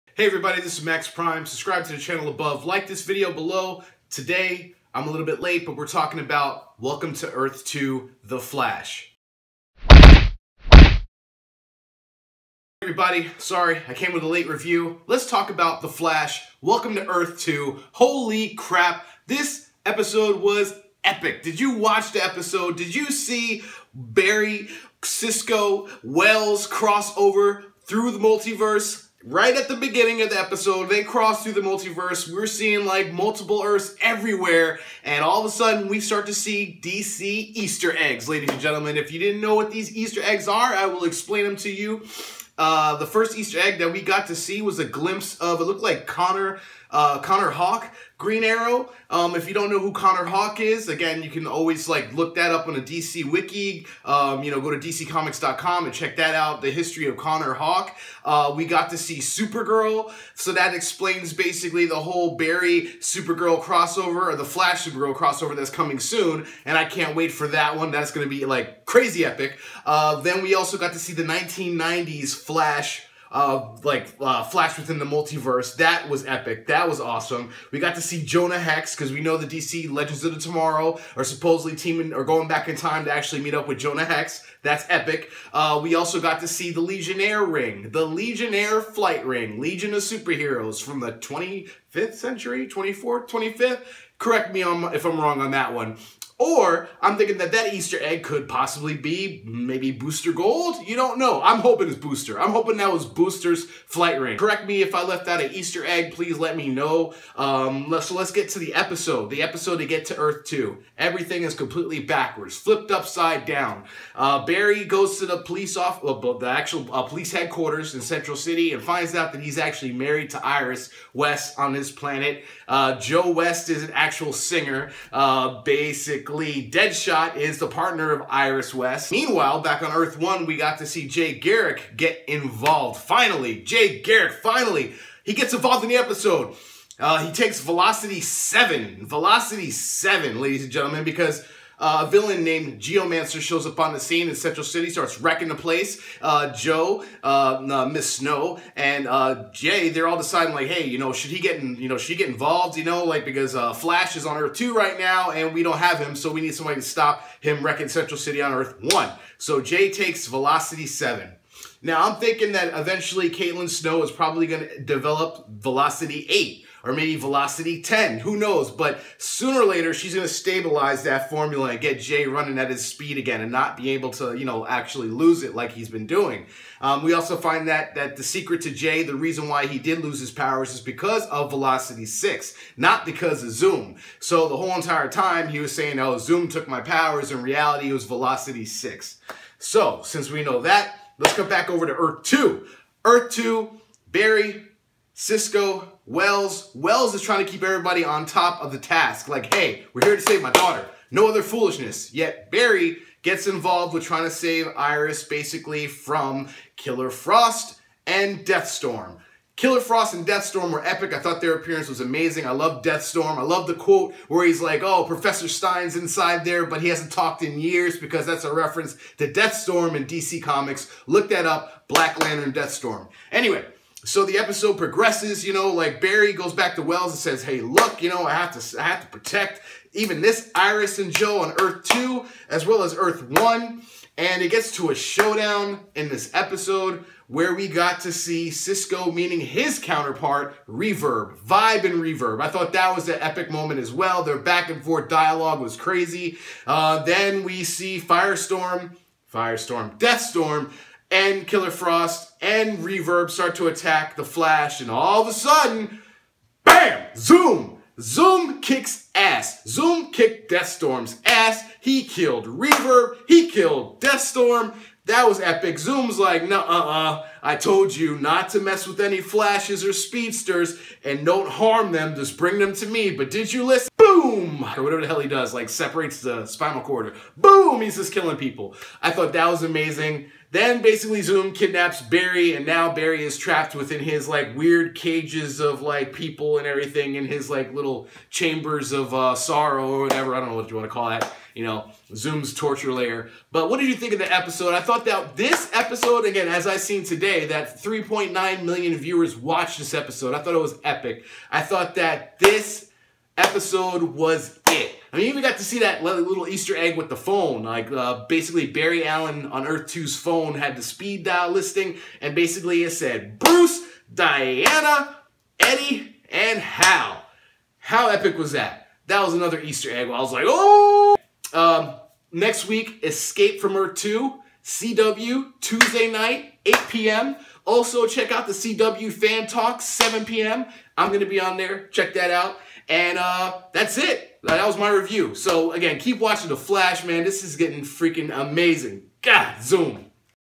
The Flash Season 2 Episode 13 "Welcome to Earth 2" Review!
This is my review of Welcome to Earth 2 - The Flash!

Twitter.com/MaxPrimeReviews